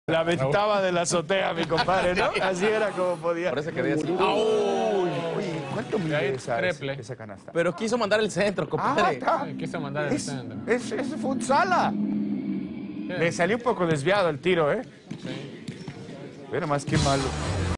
El peor centro de la historia es a su vez la mejor canasta del siglo hecha con el pie
En un partido de fútbol sala uno de los jugadores quiso enviar un centro, pero le salió tan pasado y alto que encestó un sensacional triple. Aquí la brillante (y fortuita) jugada.